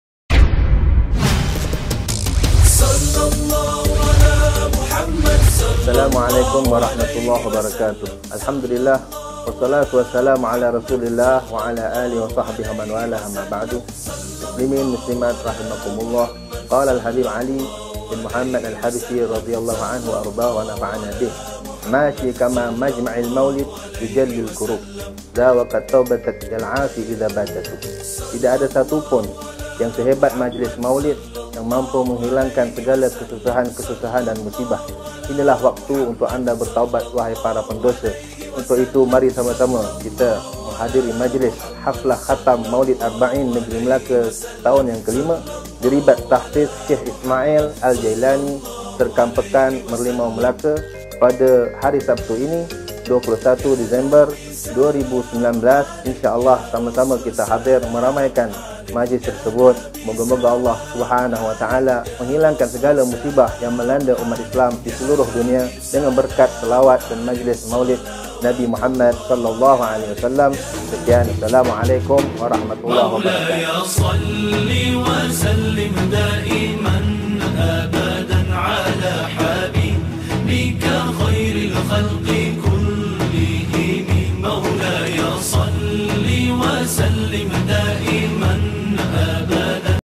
Haflah Khatam Mawlid Arbain 2019
Promo jemputan ke Majlis Khatam Mawlid Arbain 2019 Negeri Melaka. Menampilkan Tuan Guru Ustaz Noor Amali Dhiyauddin bin Hj Abdul Rahim Al-Hafiz mudir Ribat Tahfiz Sheikh Ismail Al-Jailani, Serkam Melaka.

Camera : Fujifilm X-H1
Lens : Canon EF 50mm 1:1.4